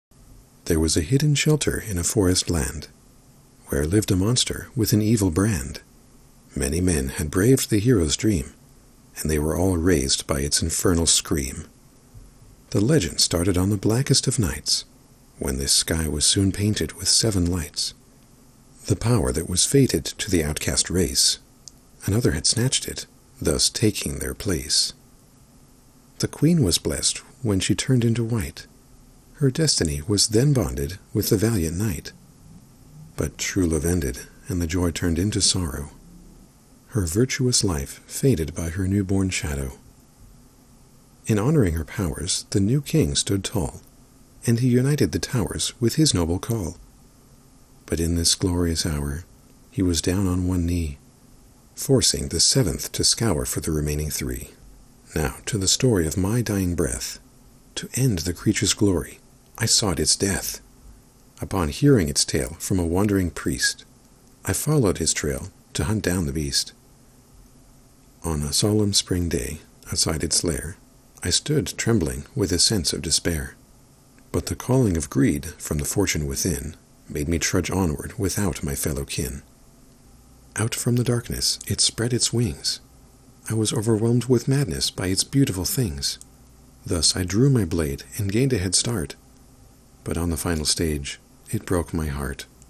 Kindle Unlimited - Fantasy Books - Poem - Fortune And Glory - The Immortals Chronicle Series
The following poem is taken from my Immortals Chronicle Book Series. It's currently free for Kindle Unlimited readers. The links for my ebook and my Blog.